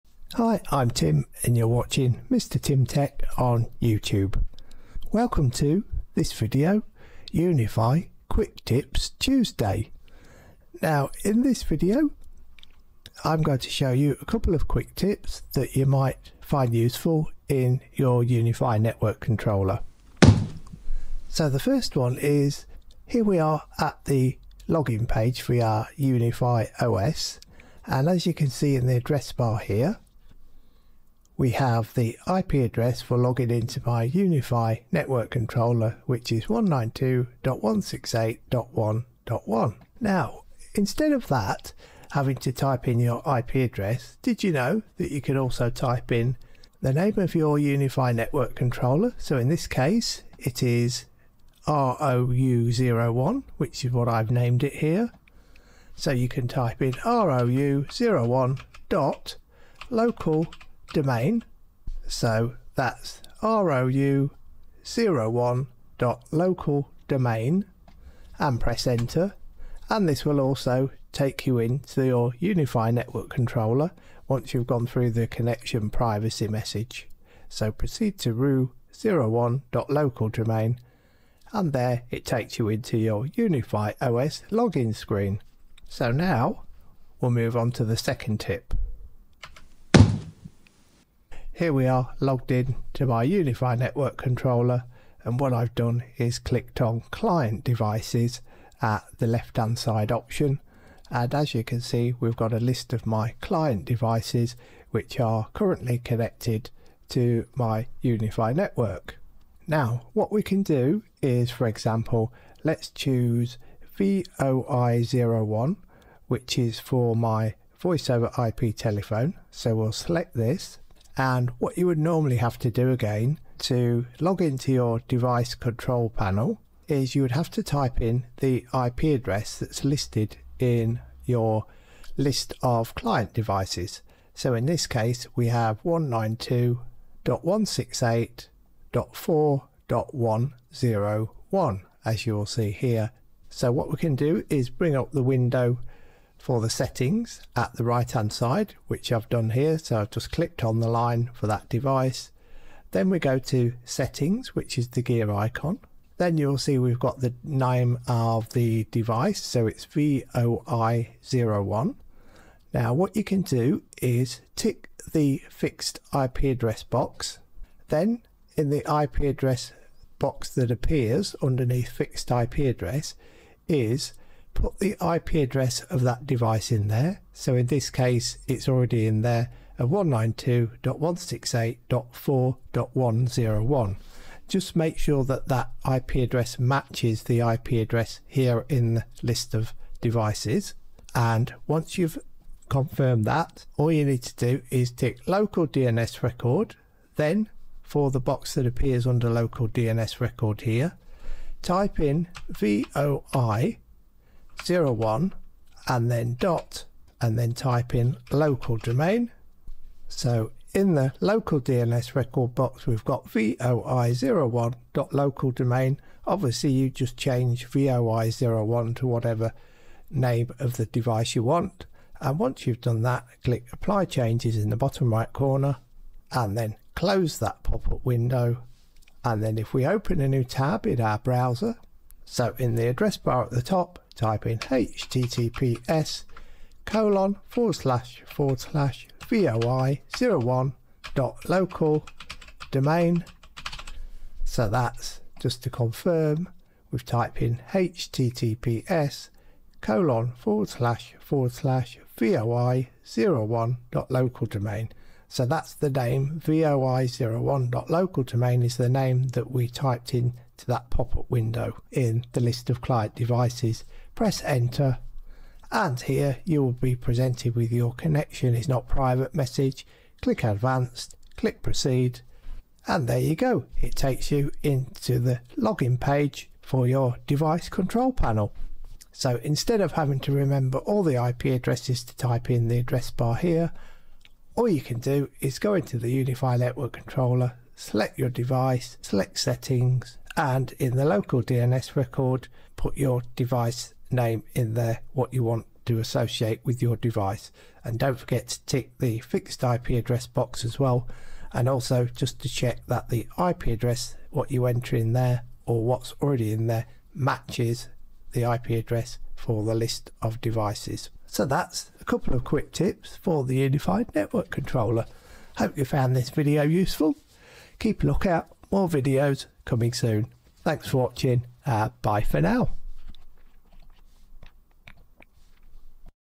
UniFi Network Quick Tips Tuesday #1
In this short video I am going to show you a couple of quick tips that you might find useful in the UniFi Network Controller so that you don't have to remember all the IP addresses for each of your client devices.

More quick tips videos will be uploaded as and when I feel there's some quick tips that you might find useful.